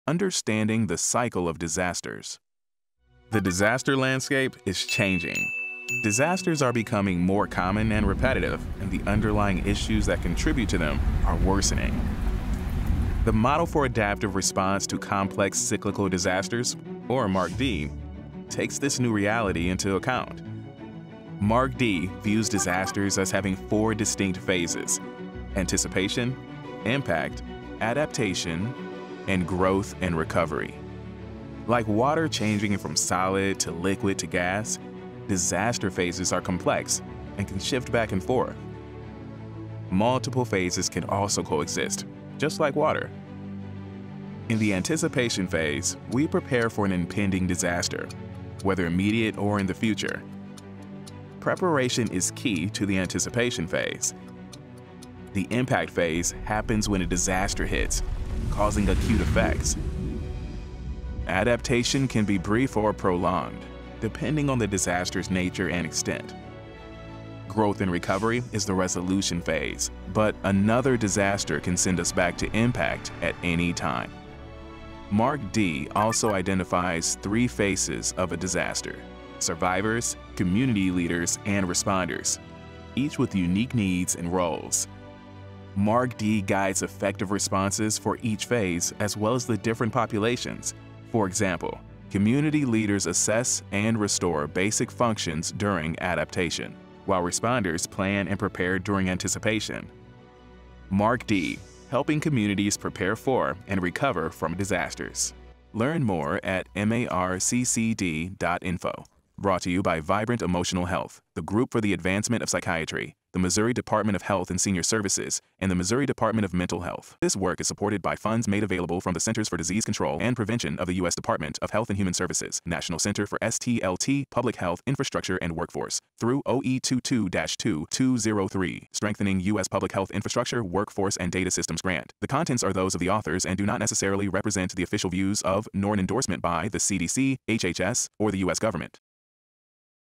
Understanding the Cycle of Disasters (Part I)
This video outlines the phases and faces that are impacted by the changing nature of disasters.